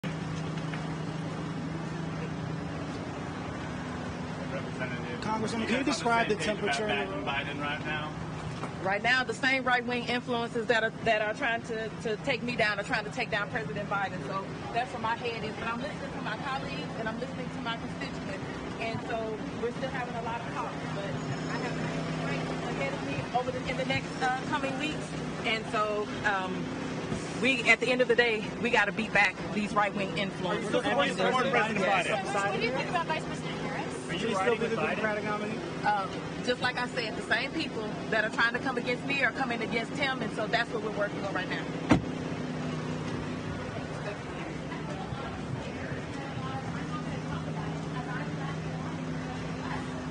Cori Bush: Same 'Right Wing Influences Trying To Take Me Down Are Trying To Take Down Pres. Biden'
Rep. Cori Bush (D-MO) discusses calls for President Biden to leave the 2024 race.